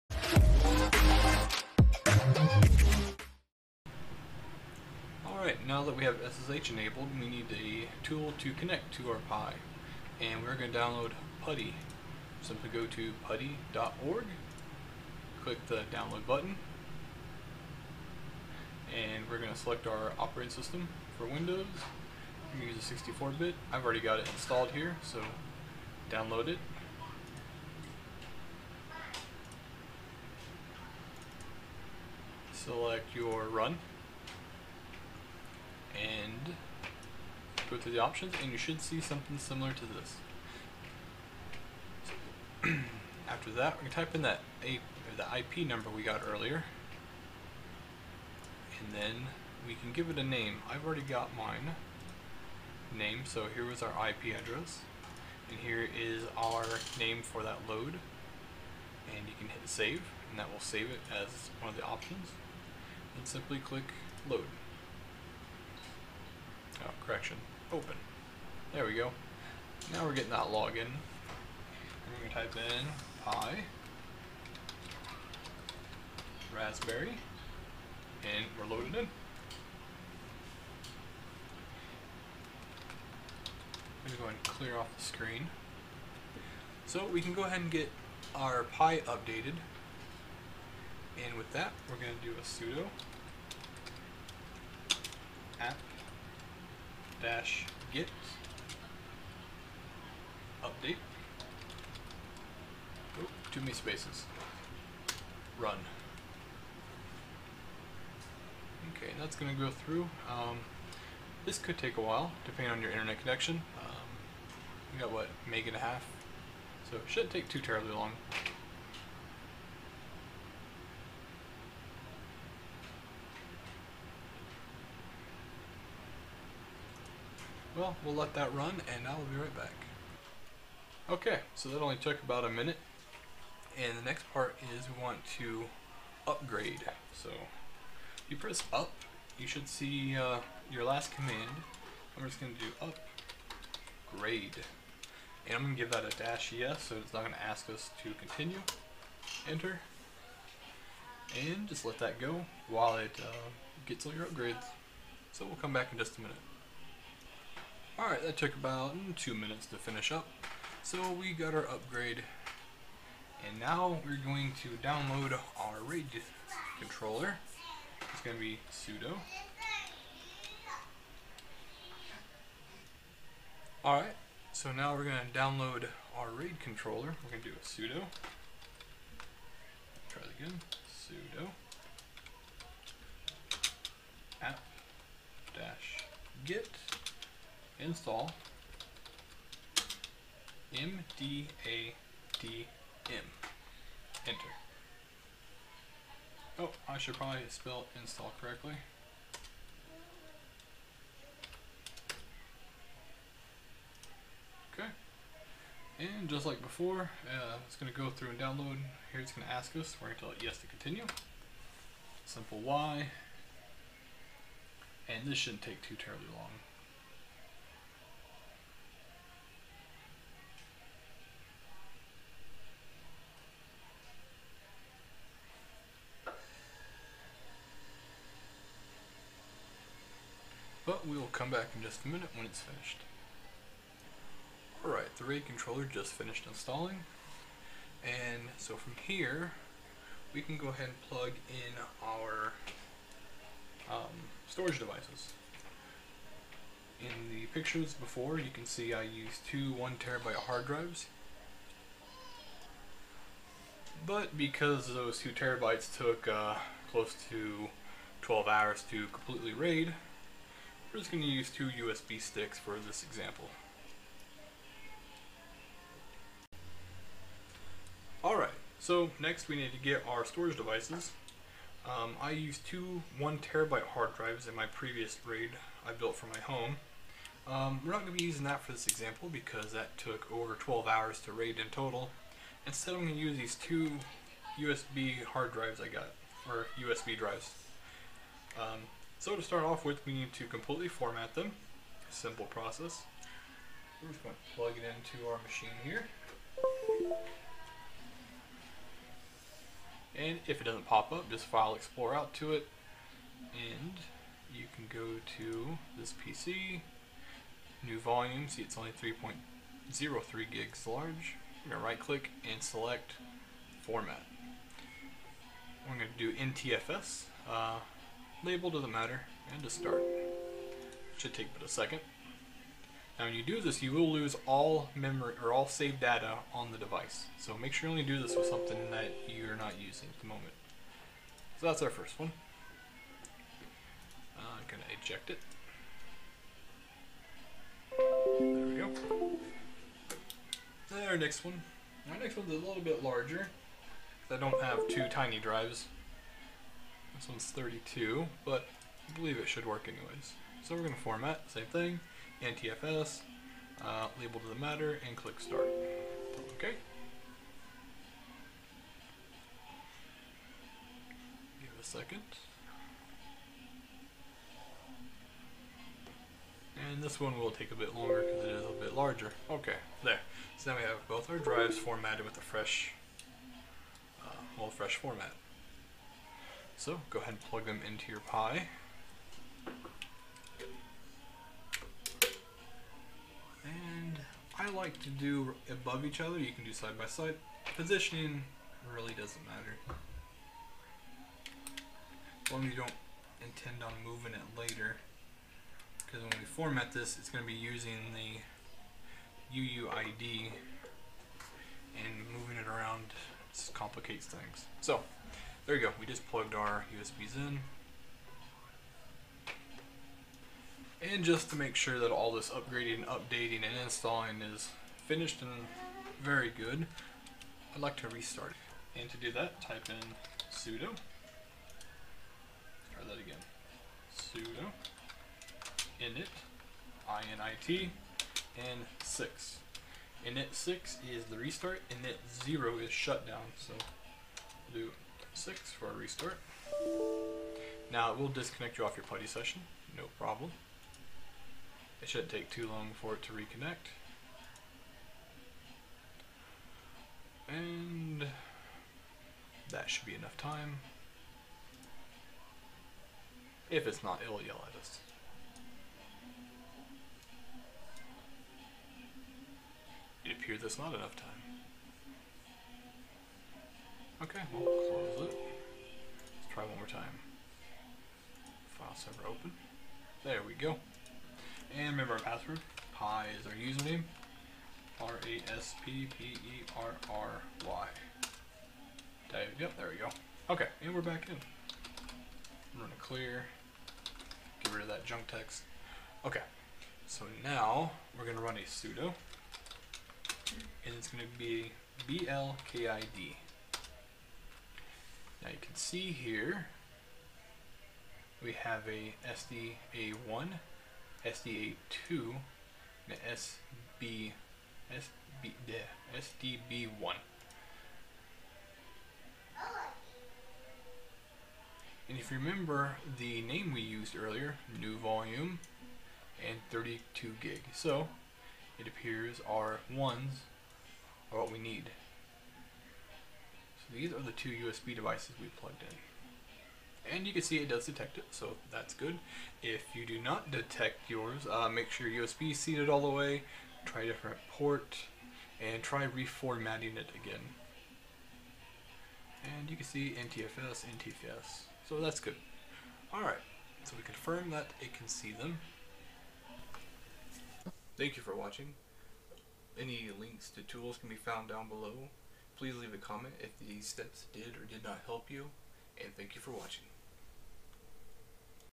Raspberry Pi Raid File Server (Part 2)
Part 2:

In this project I'll be going over setting up a Raided file server on a raspberry Pi.

The raid I'll be goginover is a Raid 1, which writes to all disks with the same information so if one disk fails a complete back up is located on the other.